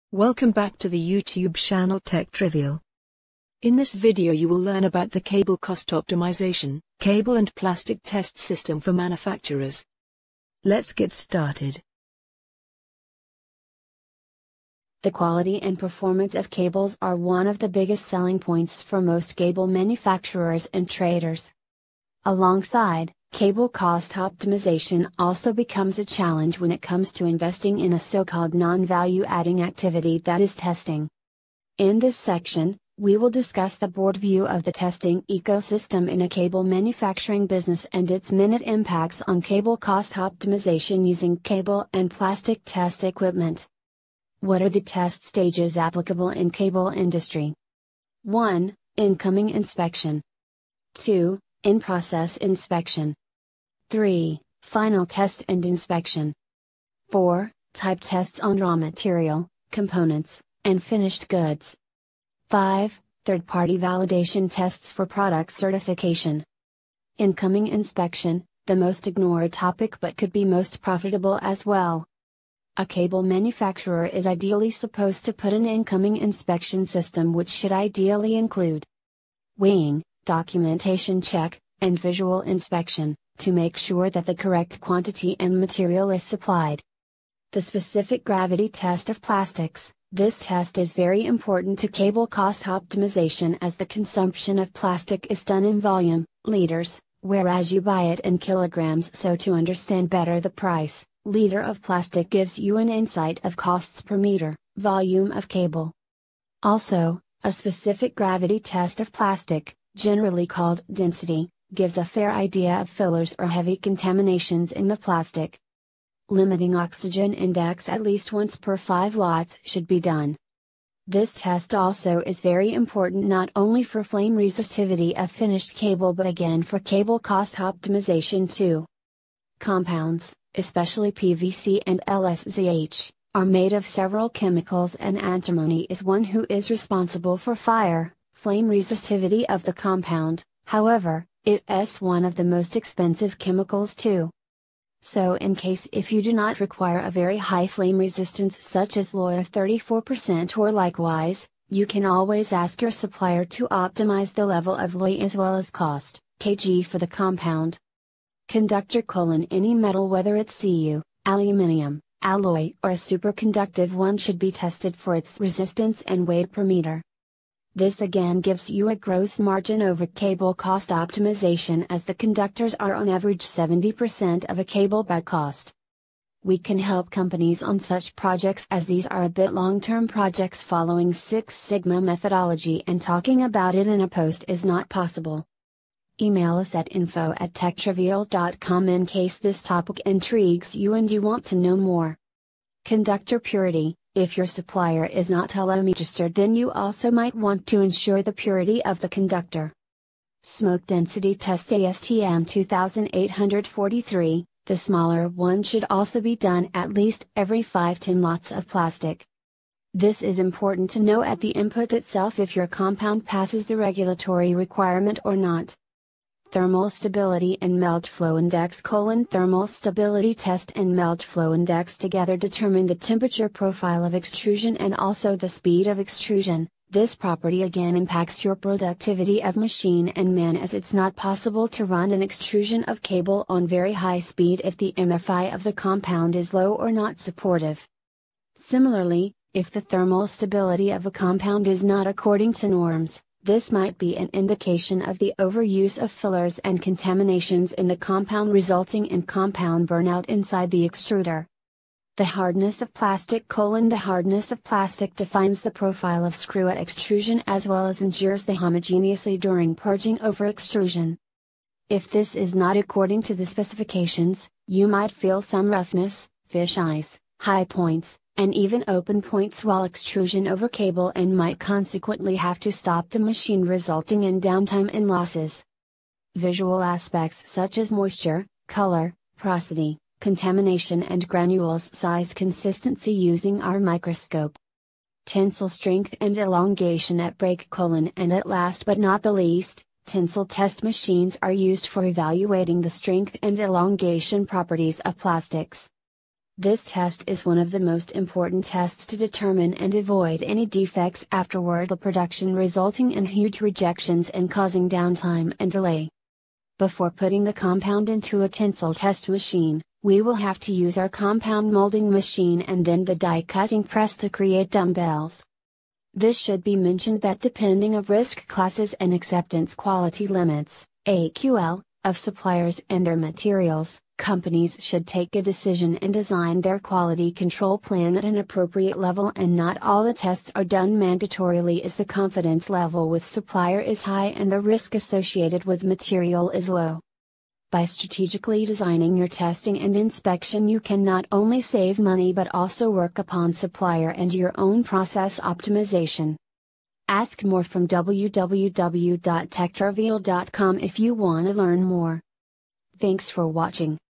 Understanding Cable manufacturing Test system with respect to cablecostoptimization
The quality and performance of cables are one of the biggest selling points for most of Cable manufacturers and traders. Alongside Cable cost optimization also becomes a challenge when it comes to investing in a so-called “non-value adding” activity that is Testing. In this section, we will discuss the broad view of testing eco-system in a cable manufacturing business and its minute impacts on cable cost optimization using cable and plastic test equipment.

Of course, all this starts from a Design-FMEA followed by Process FMEA and then a Quality control plan. Further following the sequence of APQP.

is giving you a generic Design FMEA specifically created for cable in excel template and you can always edit and enhance it as per your requirements.

Get a free editable (excel) copy of D-FMEA for Cable by following these simple steps:-